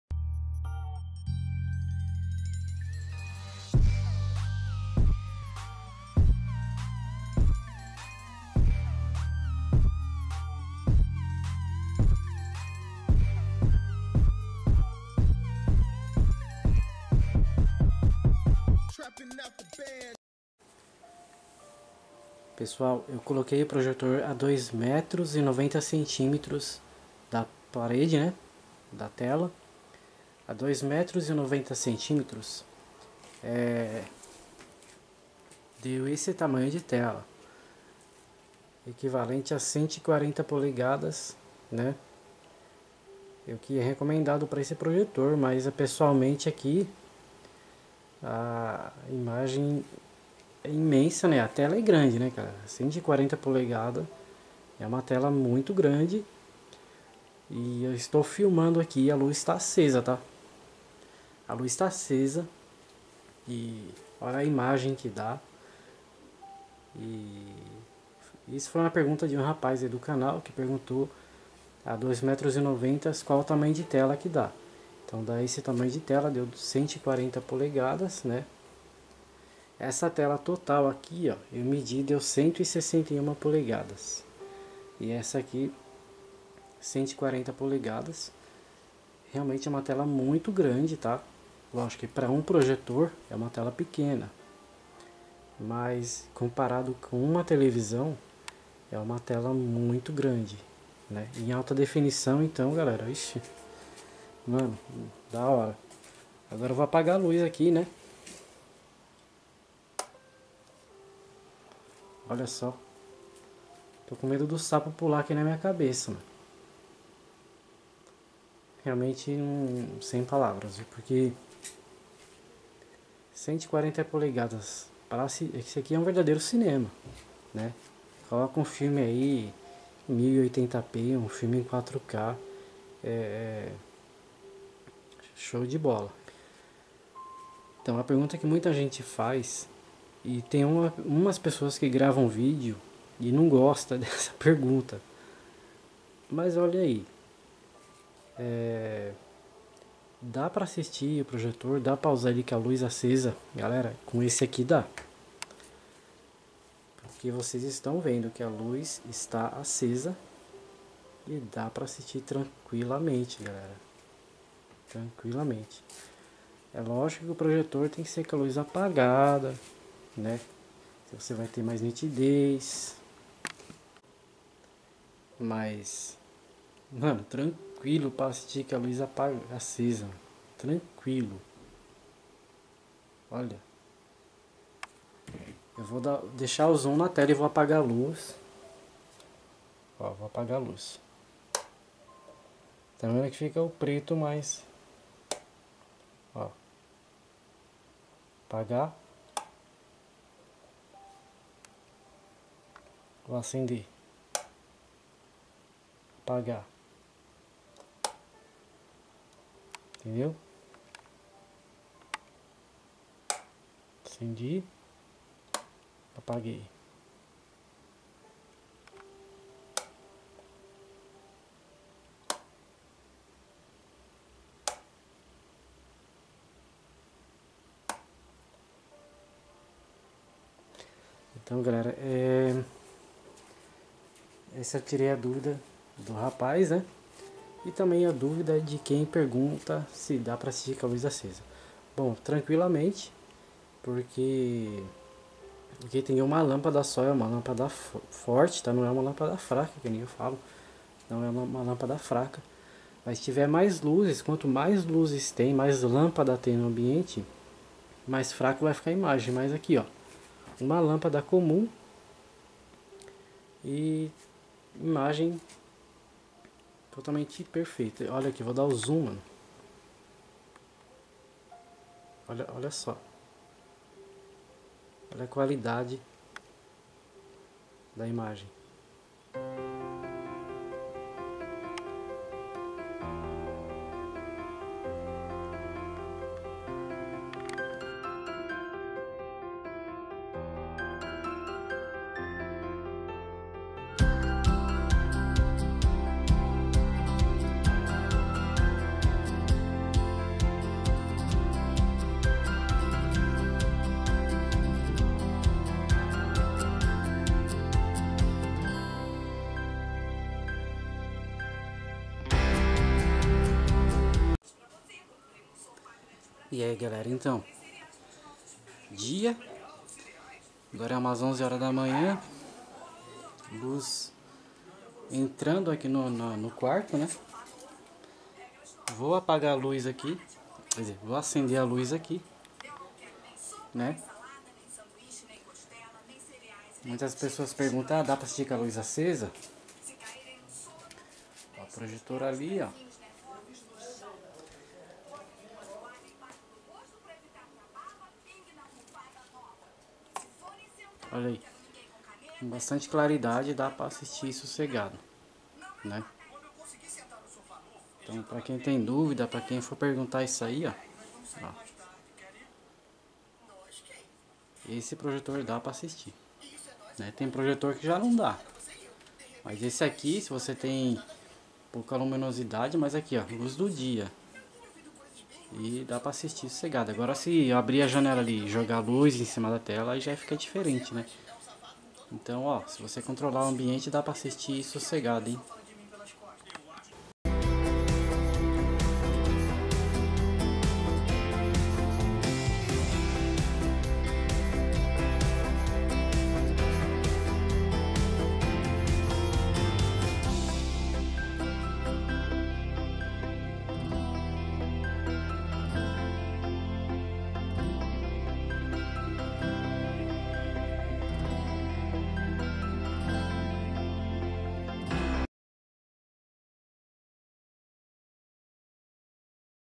140 polegadas com projetor HU70LA 4k a 2,90 centímetros da parede
Canal de entretenimento e dicas tecnológicas